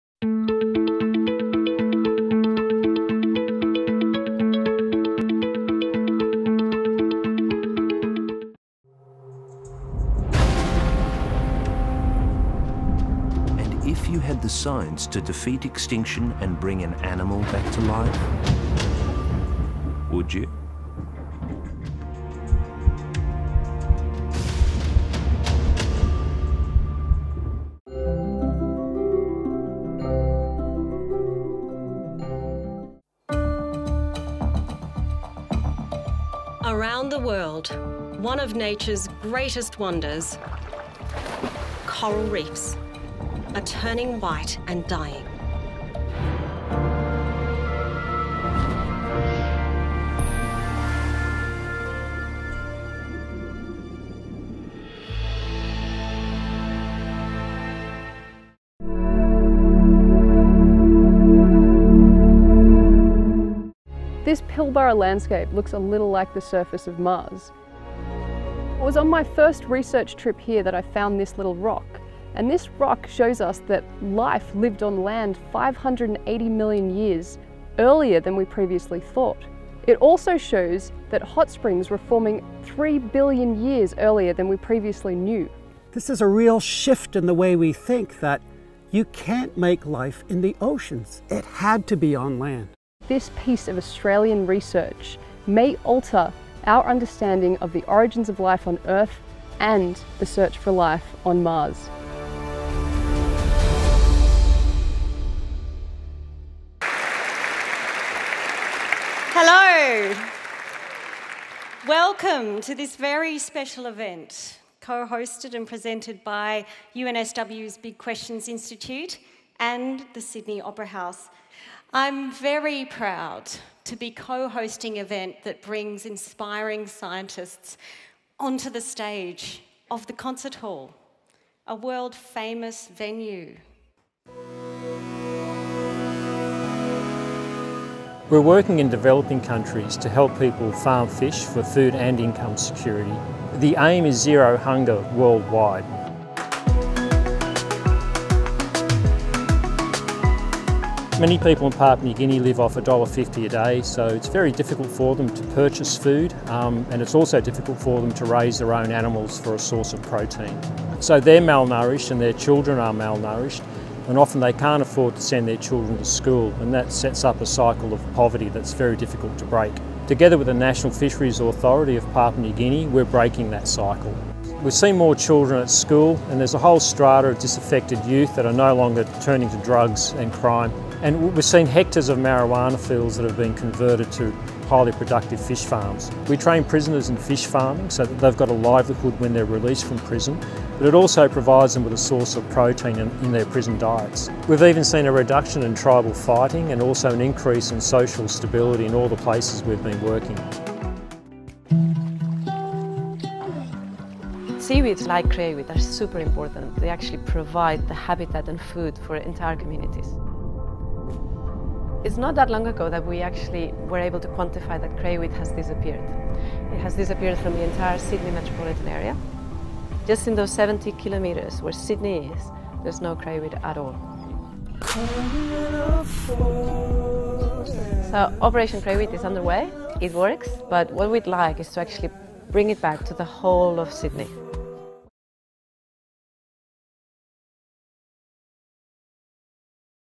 BEES 2017 Highlights: The National Voice for the Natural World (shorter version)
2017 Highlights of School of Biological, Earth and Environmental Sciences.

Dean of Science Professor Emma Johnston: hosted episode of ABC TV’s Catalyst on new ideas to save the Great Barrier Reef.

Professor Martin Van Kranendonk and PhD candidate Tara Djokic: on stage of Opera House for Life on Mars event and found the oldest evidence of land-based life.

Professor Mike Archer: documentary on de-extinction project broadcasted on ABC.

Associate Professor Jesus Sammut: aiming zero hunger worldwide by starting a fish farming in PNG.

Professor Richard Kingsford: Wild Deserts project launched by NSW Environment Minister to reintroduce bilbies, burrowing bettongs, and five other native mammals that are extinct in NSW into large, predator-free exclosures in the north west of the state.

Operation Crayweed team: won a NSW Government Green Globe Award UNSW.